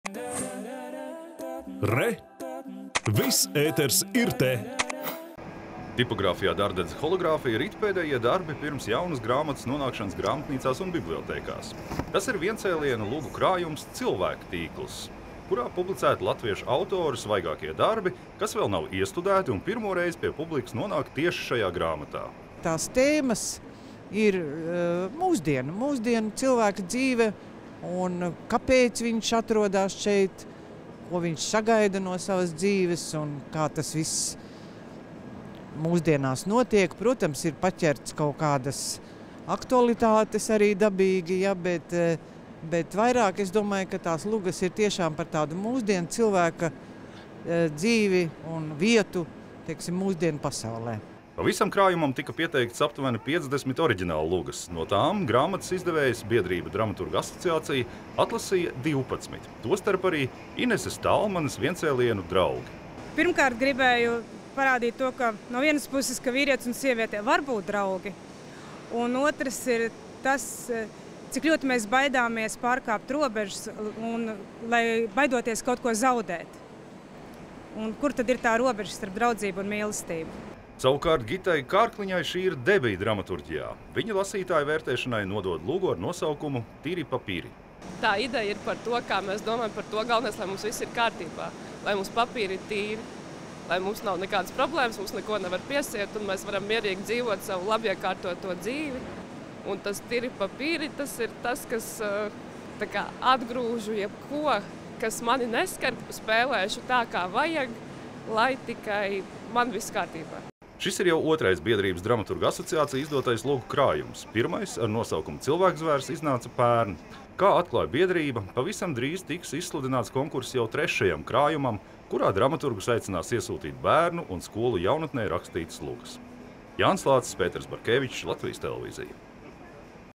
“Cilvēktīkls” – lugas par mūsdienu cilvēkiem
Pirms gada pie lasītājiem nonāca latviešu oriģināldramaturģijas krājums “Cilvēkzvērs”. Tajā tika apkopoti vēl neiestudēti un nepublicēti latviešu autoru darbi. Saglabājot šo uzstādījumu, šobrīd klajā nāk jau otrais lugu krājums. Tajā publicēti divpadsmit dramaturgu darbi ar vienojošu nosaukumu “Cilvēktīkls”. Pēdējos mirkļos pirms krājuma nonākšanas pie lasītājiem ar dažiem autoriem tikās Jānis Lācis.